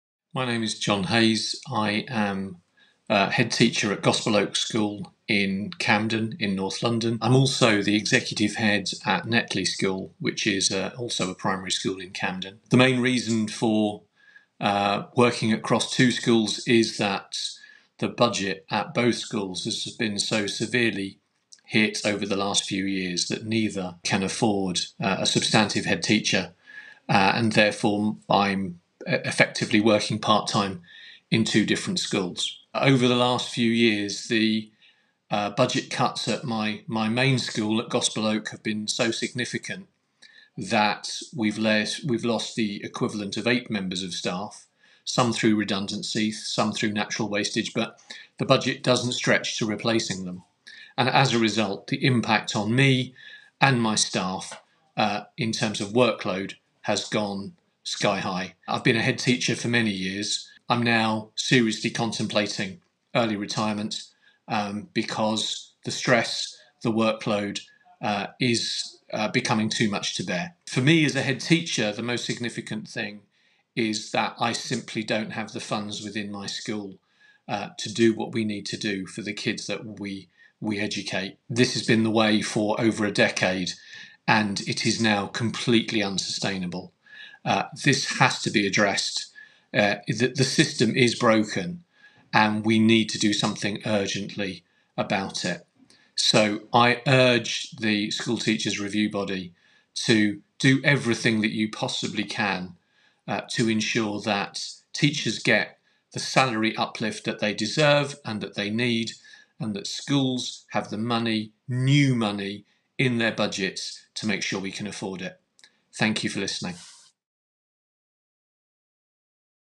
Head teacher John - STRB submission
NEU members gave evidence to the school teachers’ review body (STRB).

Head teacher John Hayes is urging the committee to tell Govt to ensure schools get new money to fund teacher pay rises.